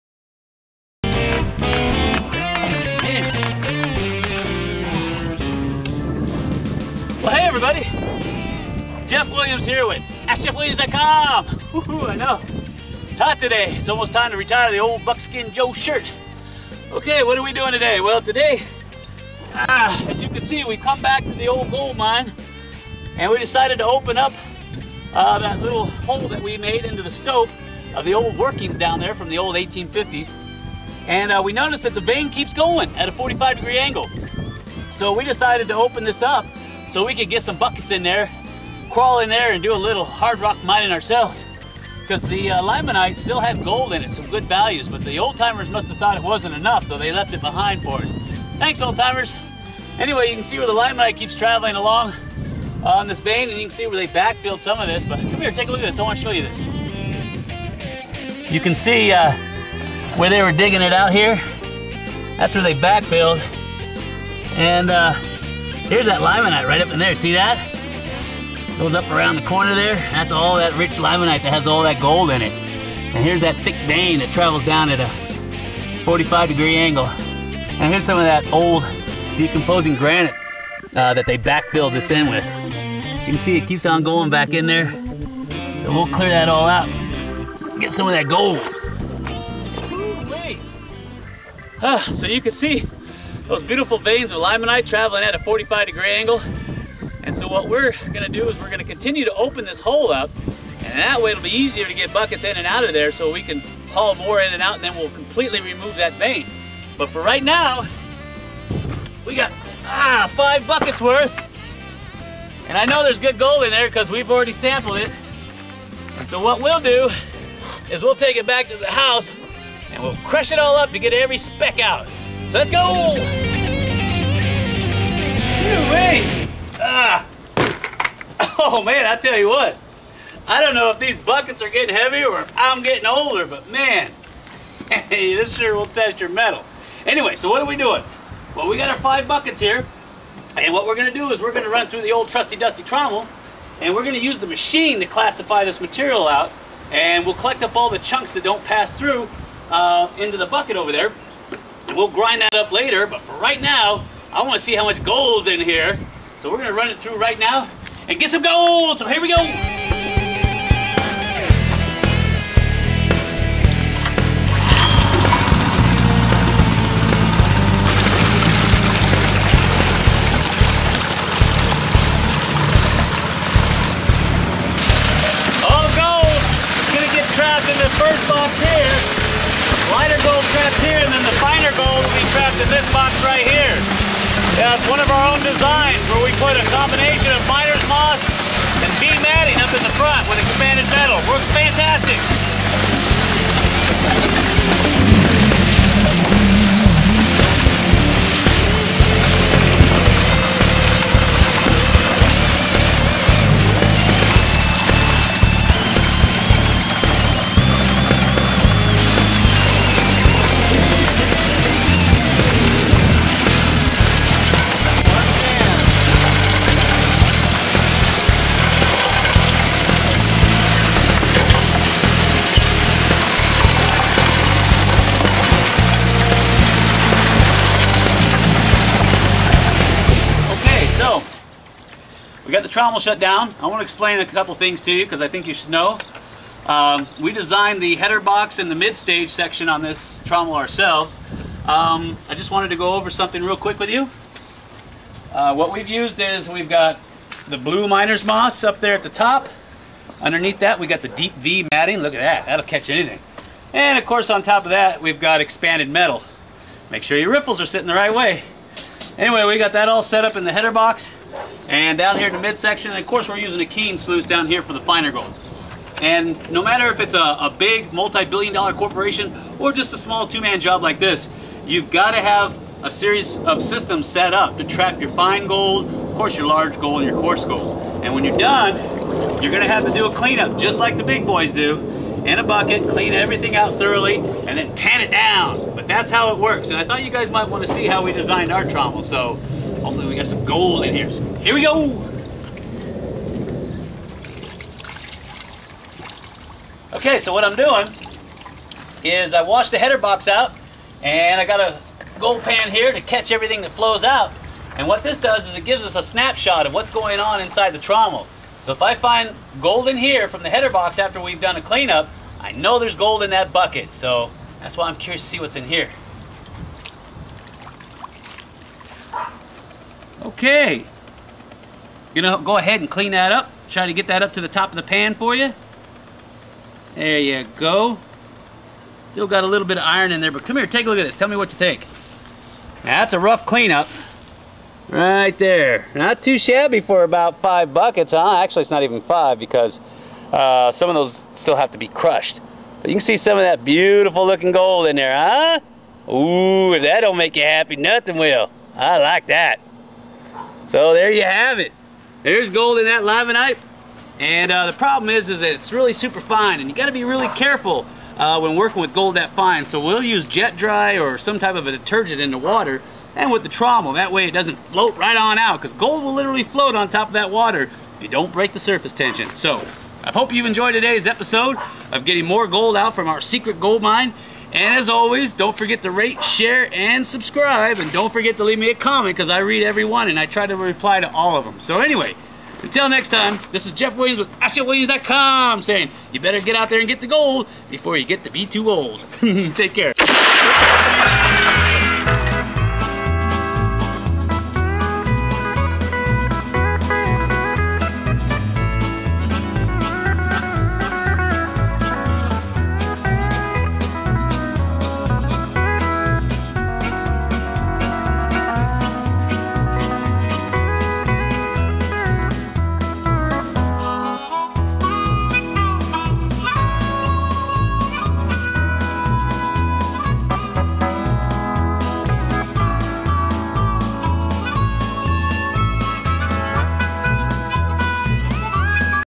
SECRET GOLD MINE !!! Finding more Gold in Limonite .
We return to one our Secret Gold mines a find more Gold in a new vein of Limonite that was left behind by the Old Timer's. First run produces Gold so next we will run the material thru a mill to get more Gold.